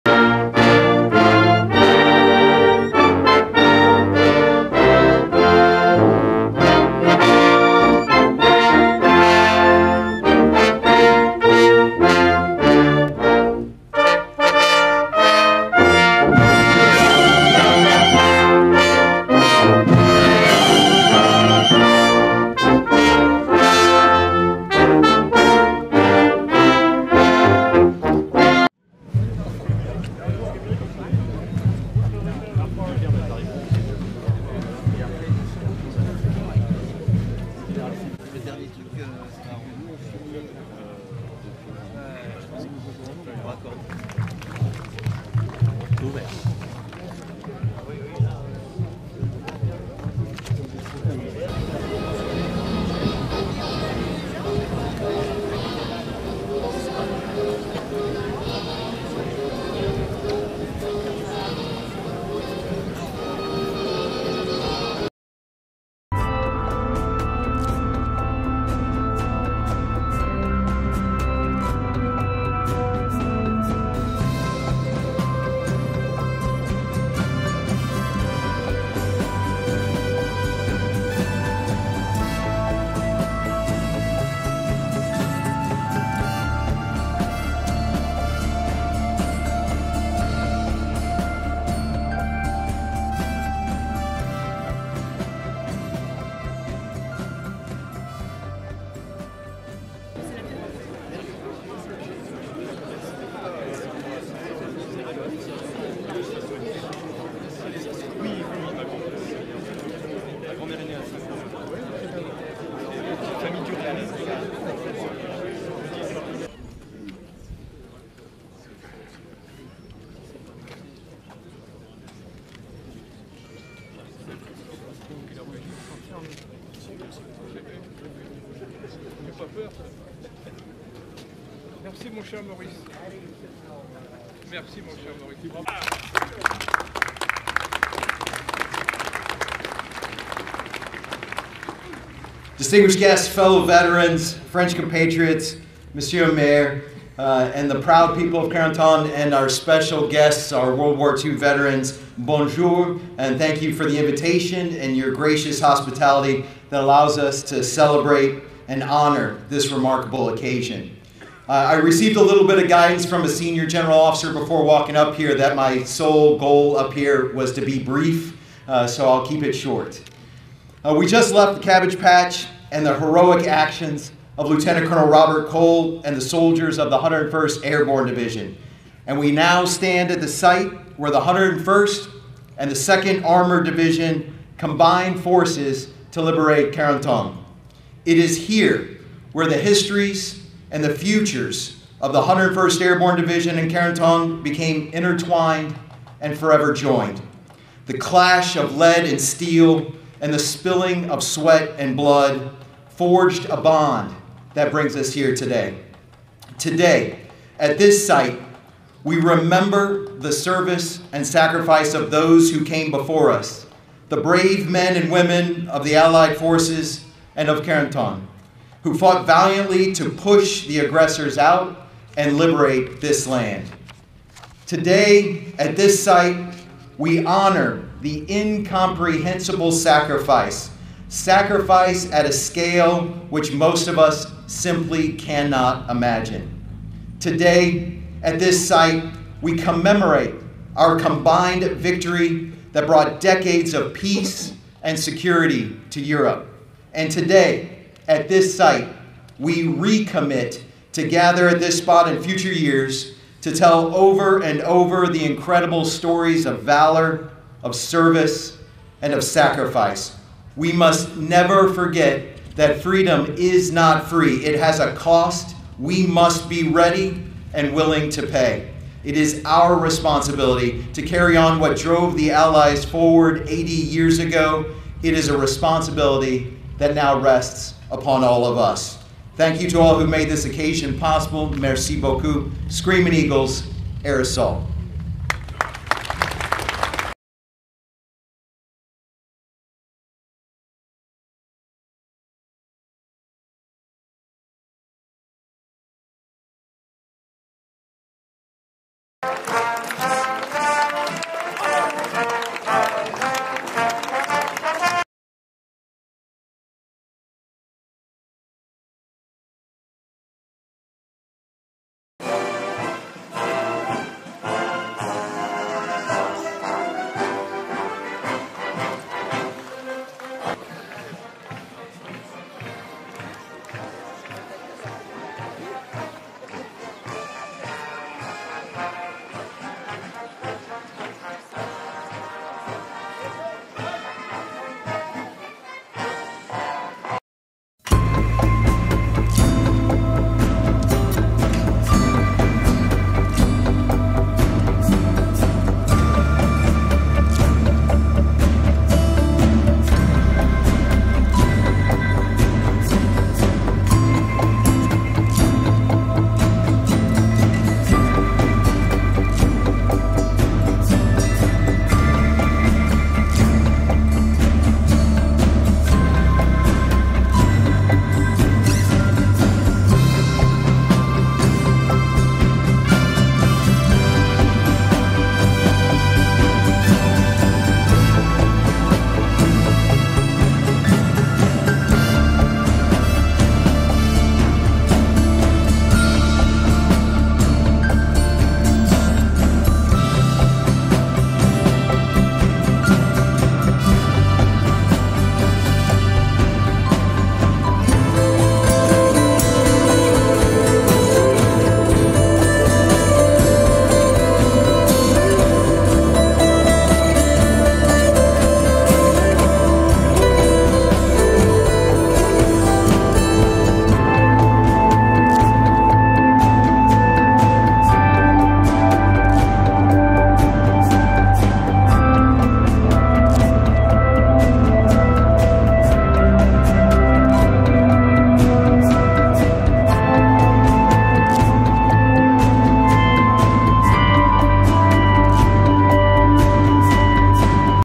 101st Airborne General's Inspiring Speech on D-Day Anniversary in France
Join us as we witness the powerful and inspiring speech delivered by Maj. Gen. Brett Sylvia, the Commanding General of the 101st Airborne Division (Air Assault), on the solemn occasion of the D-Day Anniversary in France. This momentous event took place during the Signal Monument Memorial Ceremony in Carentan, where U.S. and Allied Forces, World War II Veterans, and Distinguished Guests gathered to honor the heroes of the past. Maj. Gen. Sylvia's words resonate with pride, honor, and gratitude, reminding us of the sacrifices made on that historic day. Watch as history comes alive through his heartfelt address.

DDay 80 Signal Monument Memorial Ceremony B-roll
FRANCE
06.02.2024

The Commanding General of the 101st Airborne Division (Air Assault), Maj. Gen. Brett Sylvia, gives his remarks in front of U.S. and Allied Forces, World War II Veterans and Distinguished Guests during the Signal Monument Memorial Ceremony on June 2, 2024, in Carentan, France. The 101st Airborne Division (Air Assault) is forever linked to the people of Normandy, especially Carentan. The division fought and sacrificed for the freedom and peace that exists today in France.

Film Credits: Video by Staff Sgt. Joshua Joyner
101st Airborne Division (Air Assault)